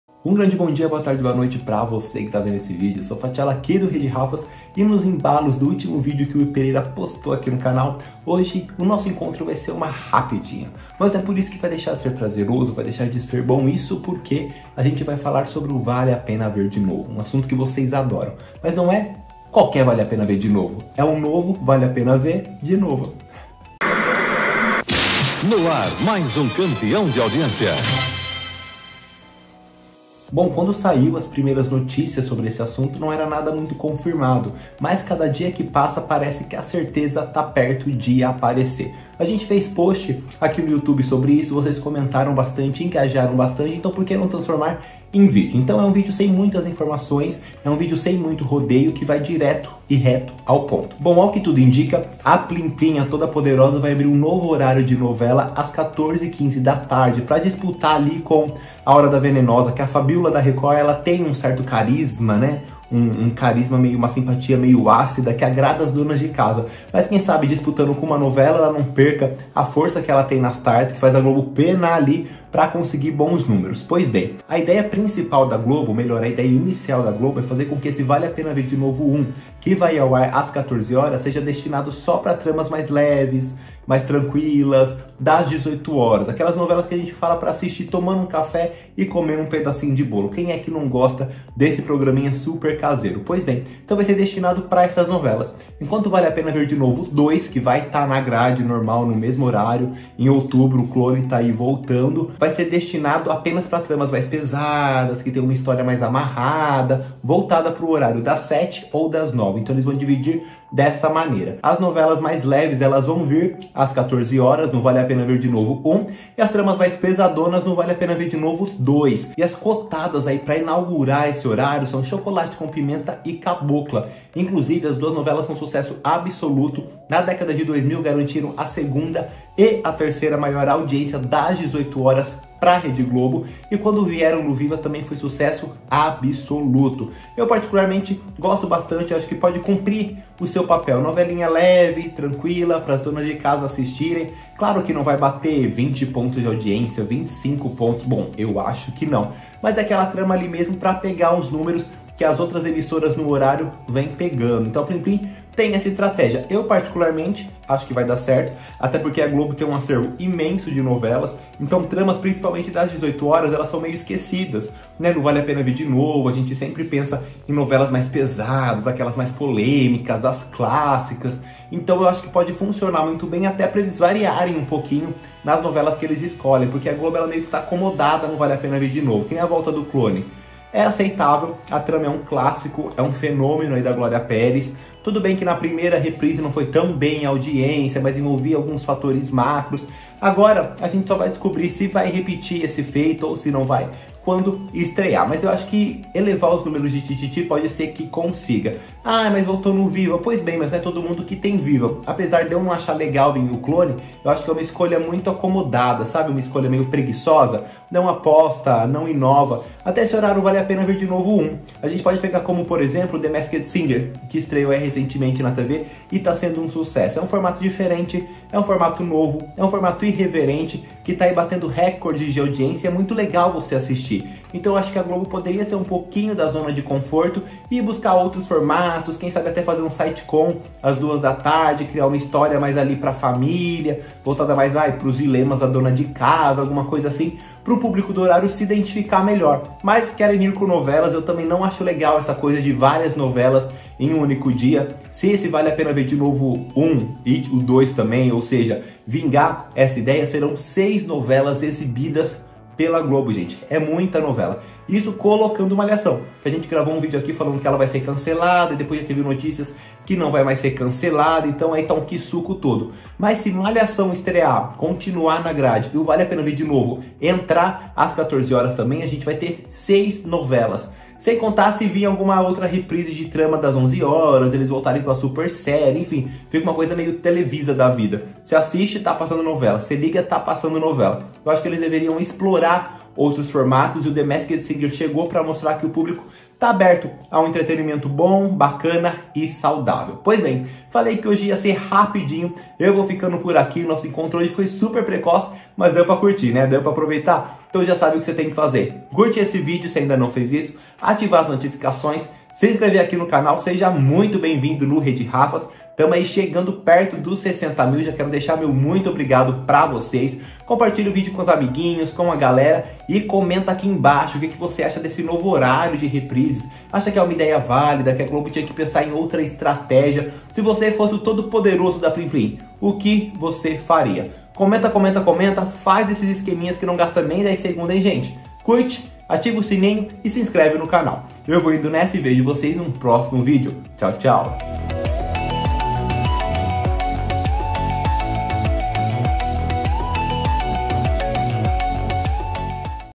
Vem aí o VALE A PENA VER DE NOVO 2?
Ao que tudo indica, a Globo pretende apostar em uma nova faixa de reprises para driblar a audiência perdida para as emissoras concorrentes, e o foco será em reprises das novelas das 18h 📺

Entre em contato conosco pelo e-mail 💻